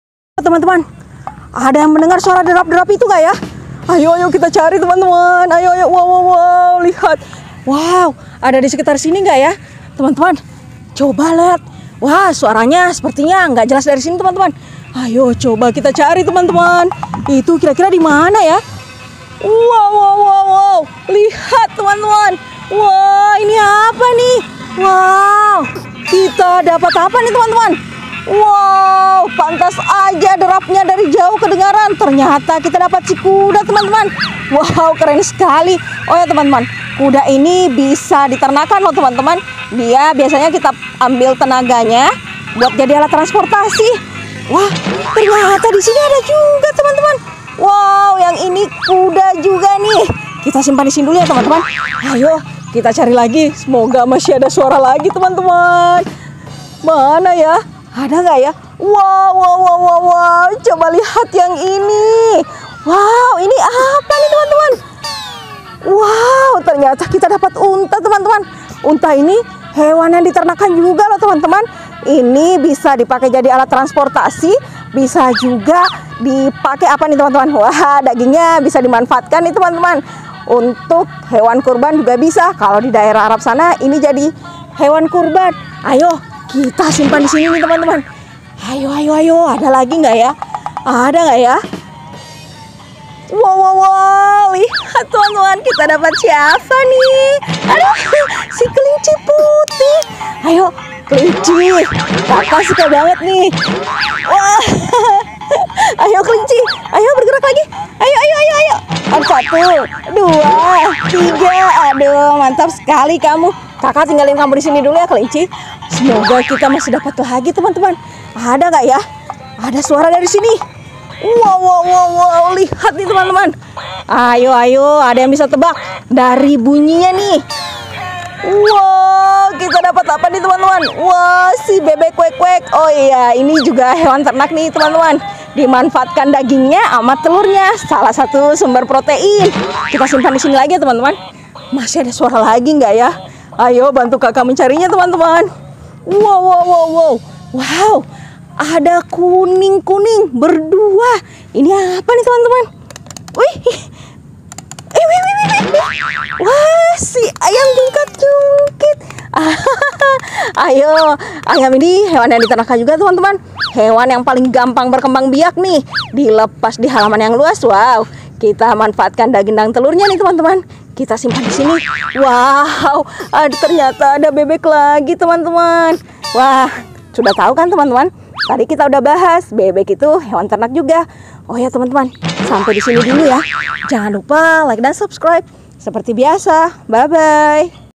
Menemukan Mainan HEWAN TERNAK Part 3 Ada Kuda Unta Kelinci Bebek Ayam
Menemukan MainanHewan Ternak Part 3 kuda Unta kelinci Kelinci putih Bebek Ayam